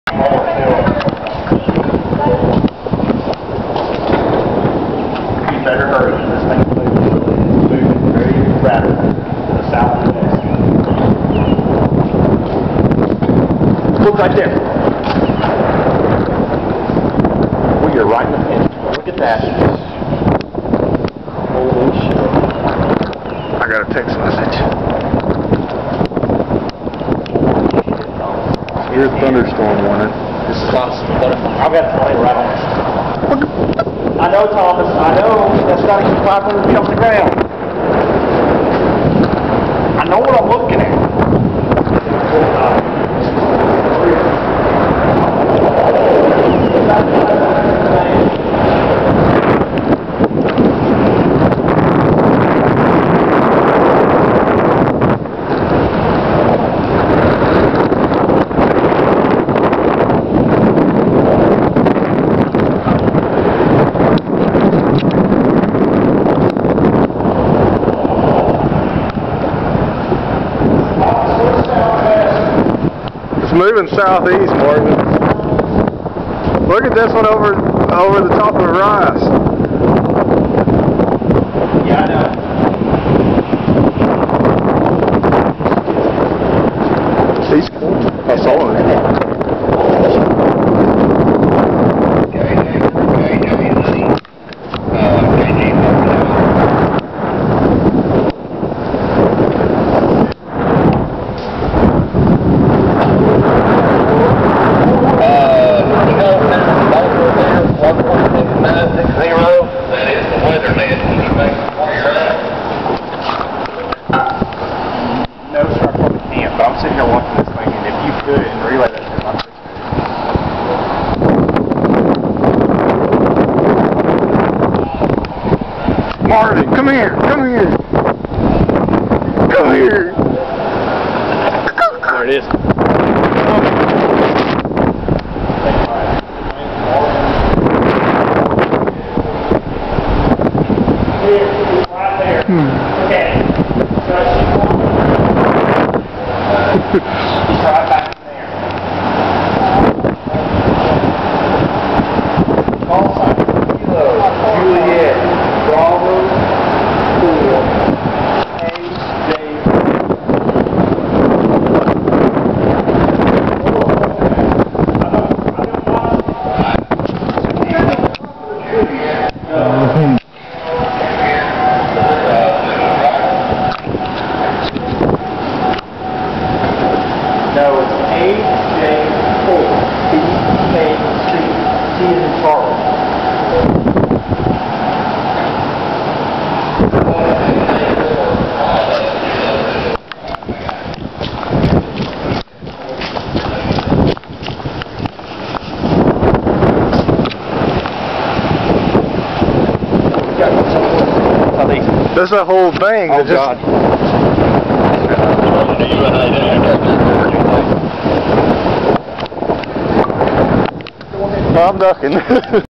Funnel clouds at UNA
This was shot from the parking deck at UNA, there was nothing more than a severe thunderstorm warning. Several funnel clouds were spotted in this video and before we started recording.